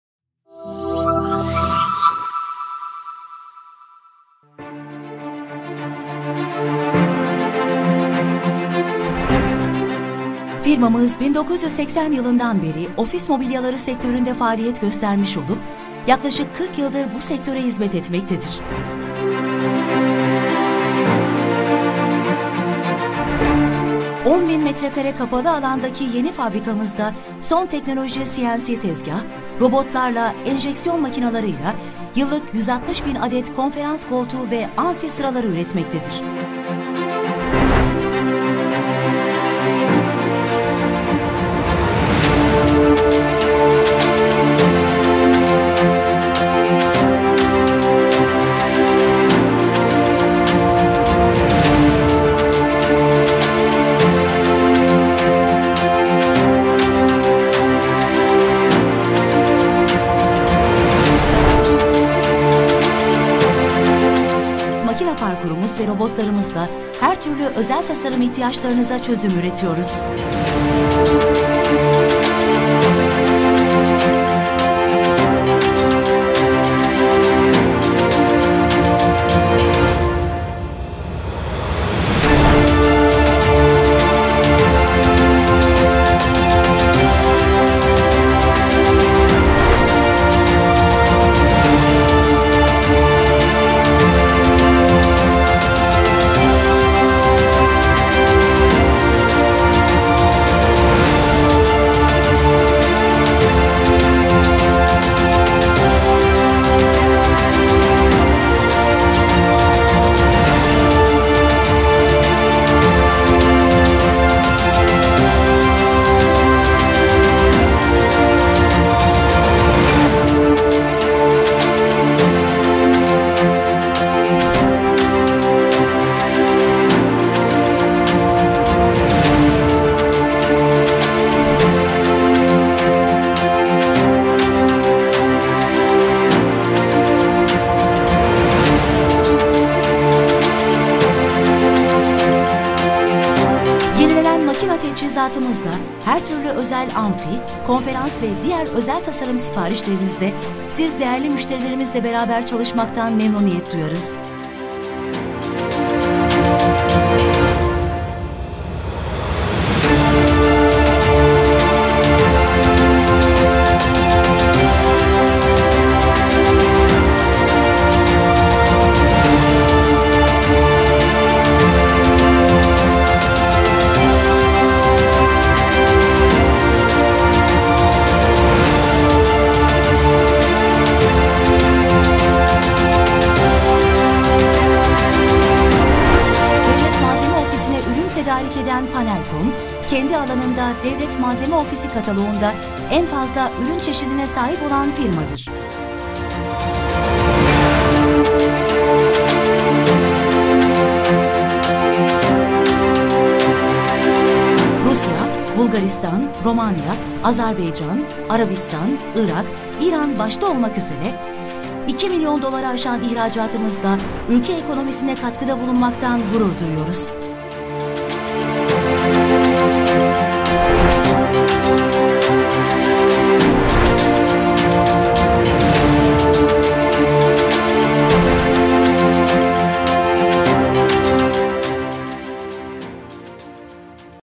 Panelkon - Tanıtım Filmi
Sektörünün lider firmalarından Panelkon için hazırlamış olduğumuz kurumsal tanıtım filmi.